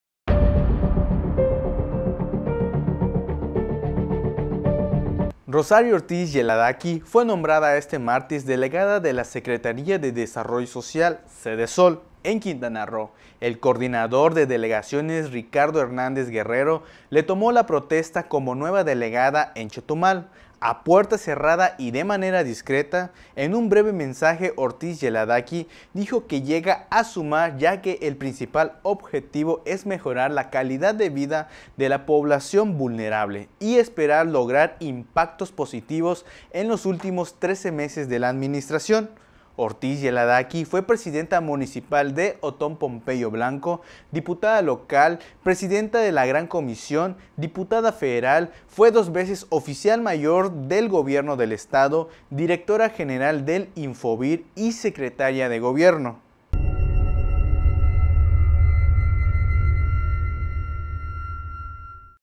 Rosario Ortiz Yeladaqui, nueva titular de la Sedesol en Q.Roo
Entérate de las noticias “EN CORTO”… Rosario Ortiz Yeladaqui, nueva titular de la Sedesol en Q.Roo…